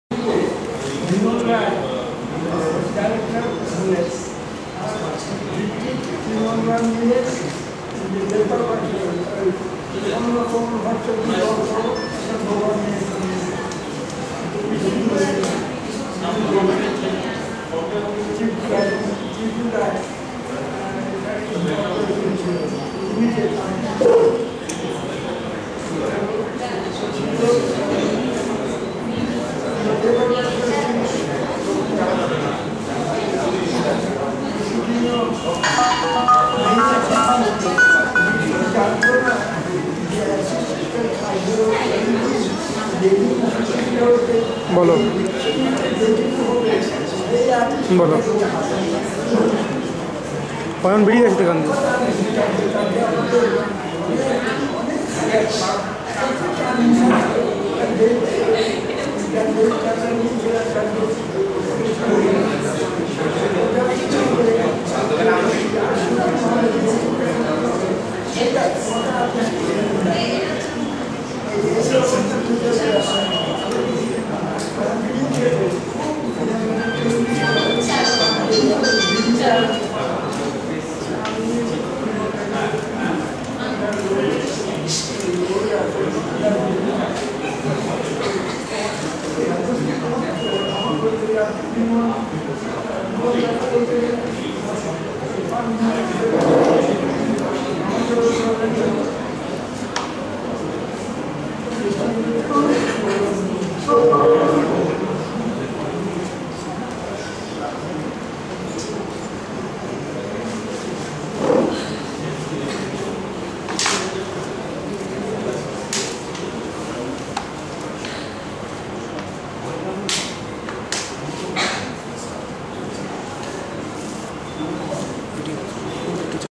Part - 2 of 6 Inauguration of the 1st ODOP - LC Photography Exhibition - 24th May, 2013.
Inaugurated by Eminent Cinematographer Shri Ramananda Sengupta and National Award Winning Director Shri Nripen Ganguly.

Gallery: CAMARENA Super-Store, E-mall (ground floor), Calcutta.